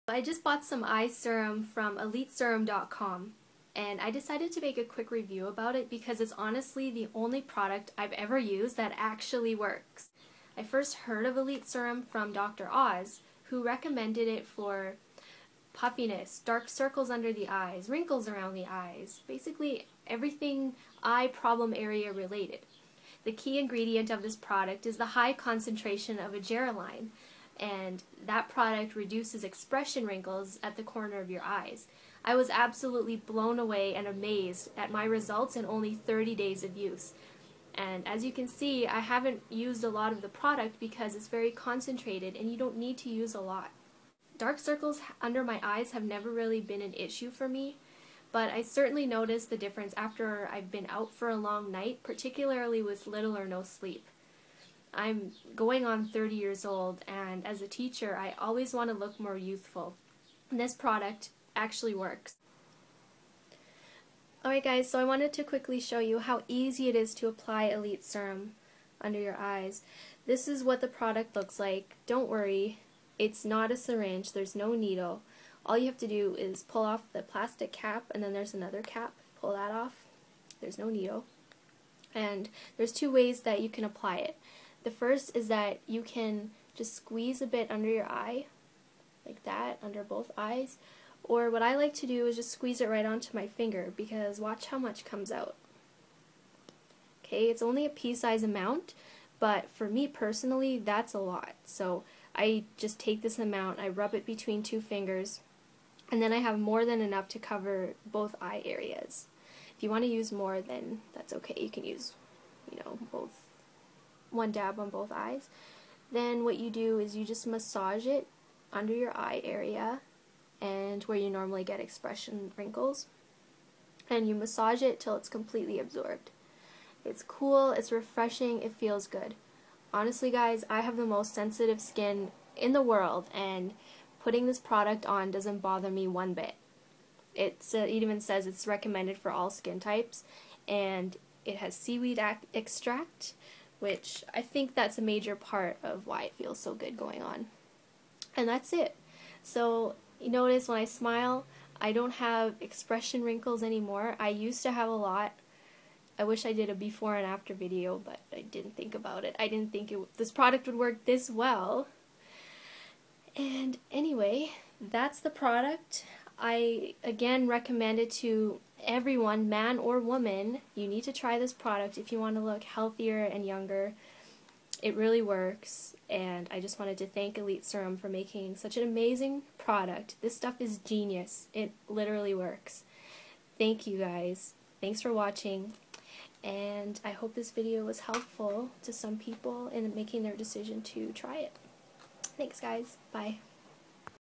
Christie's Elite Serum Review
Christie, a 28 year old public school teacher, reviews and demonstrates how to use Elite Serum in this short clip. Please keep the product reviews flowing!

Read our Full Elite Serum Review

Check it out right here

Elite Serum Rx is a product of Skin Pro International:
Skin Pro International
1602 Alton Road
Suite 597

Thanks for reading the Elite Serum Rx review.